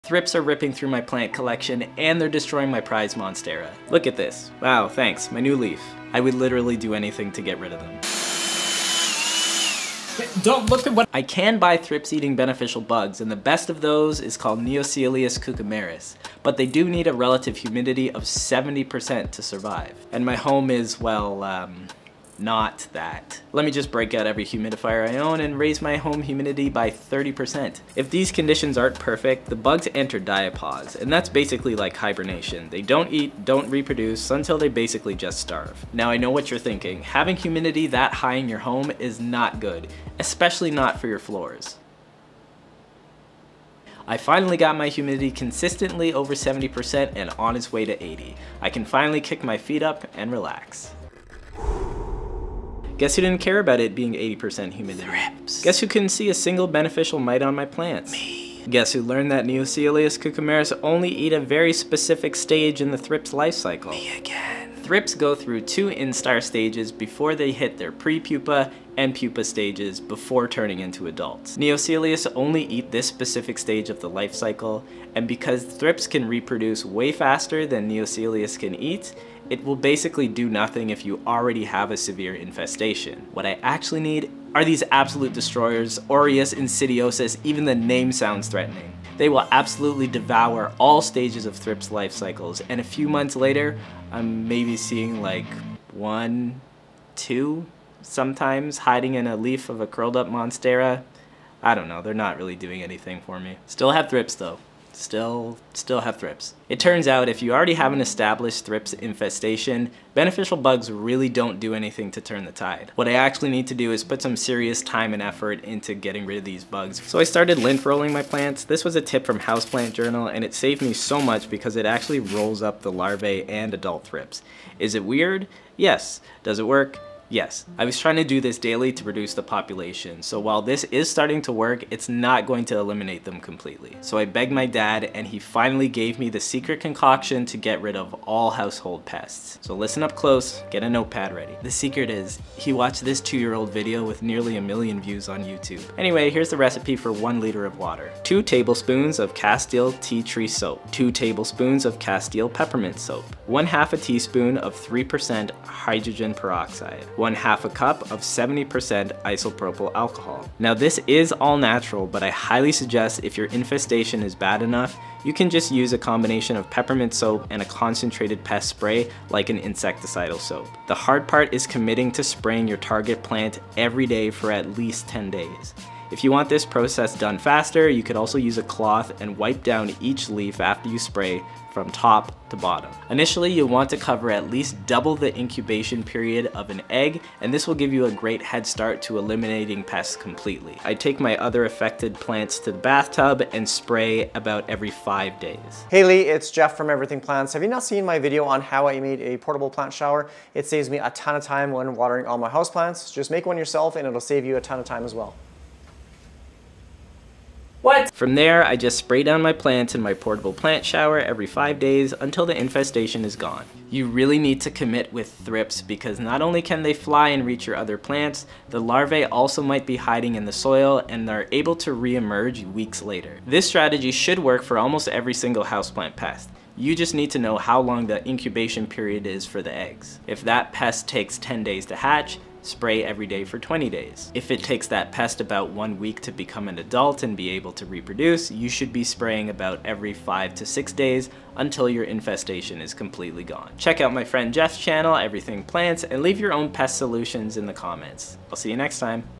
The SECRET to get rid of plant pests
It had been such a wild ride to get rid of Thrips in my plant collection. One of the worst houseplant pests you can deal with. This secret should help you finally get rid of indoor plant pests in your collection too. As long as you're consistent!

This recipe should work with most major common plant pests. Including:
Spider mites
Thrips
Mealy bugs
Aphids
Whitefly

Beneficial insects
Throughout filming this video I tested a few different beneficial bugs, the most effective for me has been:
STRATIOLAELAPS SCIMITU: These live in soil and eat thrips larvae and fungus gnat larvae. They do extremely well as a preventative.
AMBLYSEIUS SWIRSKII (in sachets): These bugs eat early instar stages of thrips and have been the most hardy to low in home humidity.

Other recommendations:
Green Lacewings
Orius incidiosus (summertime only)

Secret concoction:
In 1L/34oz of water
2 tbls castile peppermint soap
2 tbls castile tea tree soap
1/2 tsp 3% hydrogen peroxide
1/2 cup 70% isopropyl alcohol

Use the whole batch immediately.

Alternative concoction
2 tbls castile peppermint soap
(as directed) concentrated insecticidal soap

Safety: If you're using a fine sprayer the particles can get into your lungs. Because of the isopropyl alcohol content I recommend using a respirator. If you're using a concentrated insecticide you should be using full body PPE. Always follow the labels of pesticide/insecticide products.

Pump Sprayer
Castile peppermint soap
Castile Tea tree soap

Products I Recommend:

Amazon USA
Amazon Canada

I keep my plants healthy by watering and fertilizing consistently